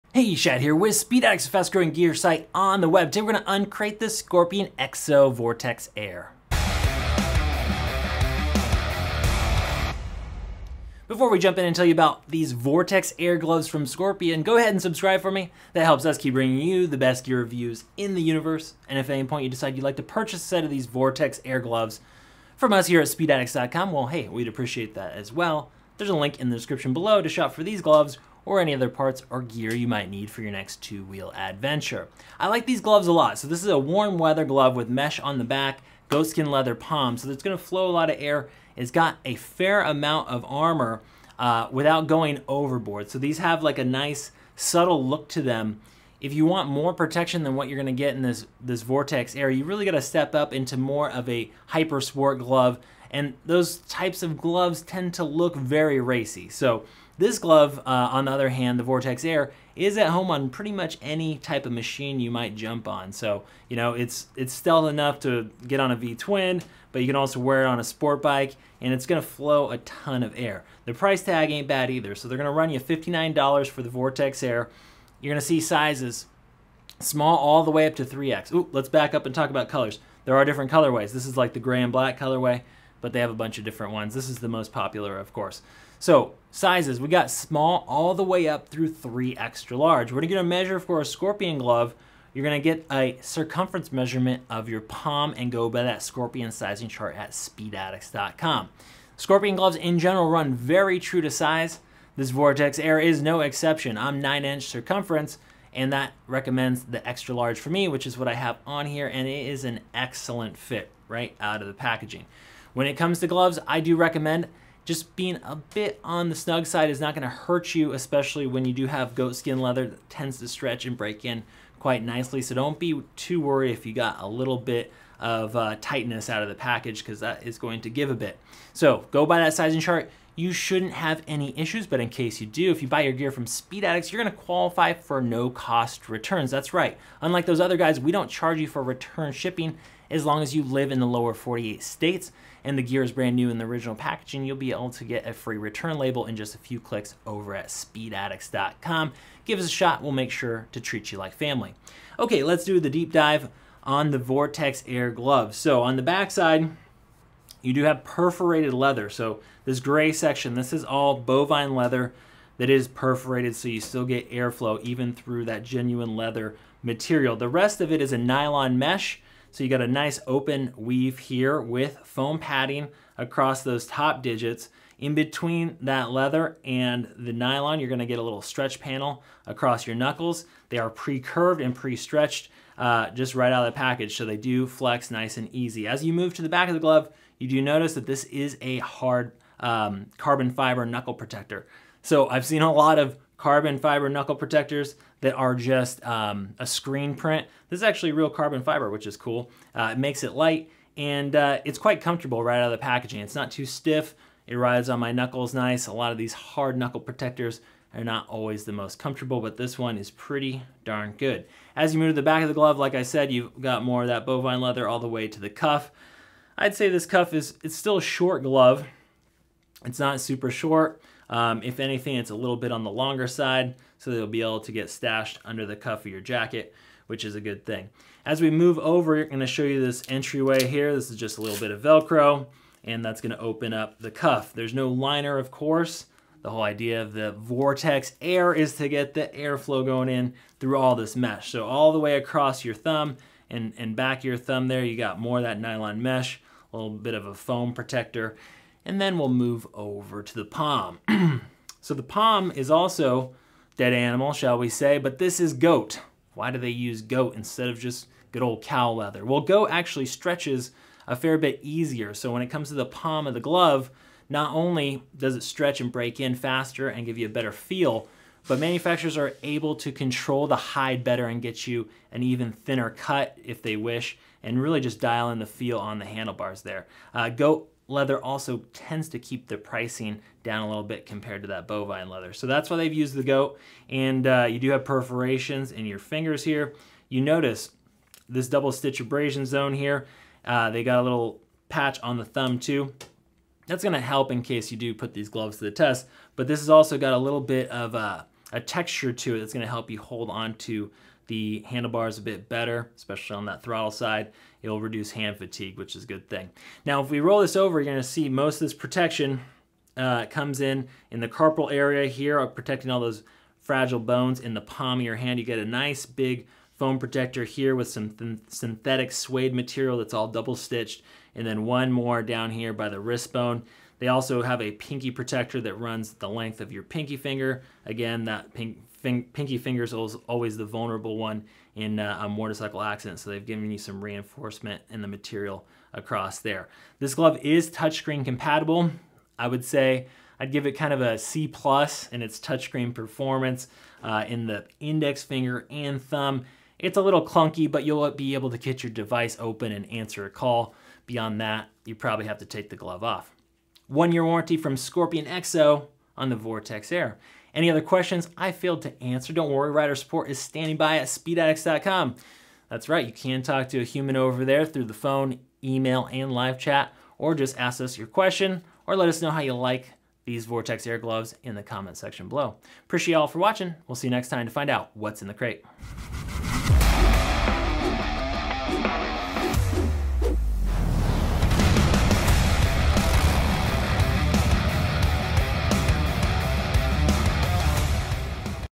Scorpion Exo Vortex Air Gloves Review at SpeedAddicts.com
SCORPION EXO VORTEX AIR GLOVES - AVAILABLE NOW AT SPEEDADDICTS.COM

You dont need to compromise on safety to get a great warm weather glove. The combination of a goatskin leather chassis, carbon fiber knuckles, and reinforced palm provide the protection, while the high flow mesh and perforated leather provide the air flow. Get the airflow you want with the protection you need from the ScorpionEXO Vortex Air glove.

Premium goatskin leather glove with durable polyester mesh provide protection and comfort
Carbon fiber reinforced Knuckle provides strength and durability
Stretch panels at knuckles for dexterity
Padded knuckles, thumb, and palm provide impact protection
Reinforced sidewall and palm add abrasion resistance
Digital embossed reinforcement on palm and thumb provide superior grip
Pre-curved finger construction for fit and reduction in rider fatigue
Touchscreen compatible fingers and thumbs
1 year warranty